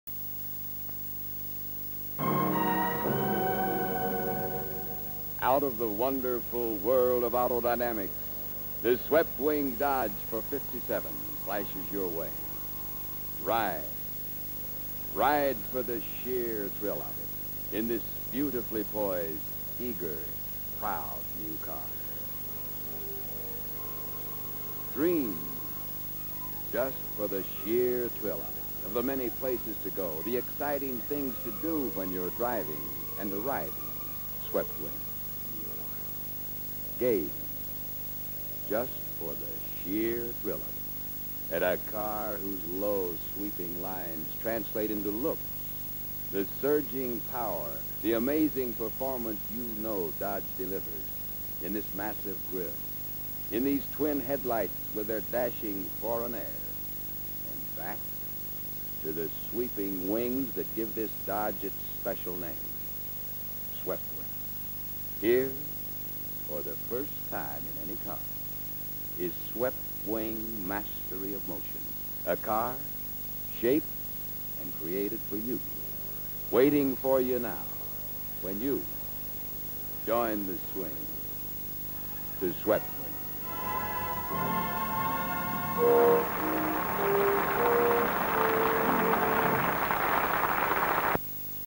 1957 3 of 3 Dodge TV Ad with Lawrence Welk
1957 Dodge TV ad featuring Lawrence Welk.
3 of 3

V. Romano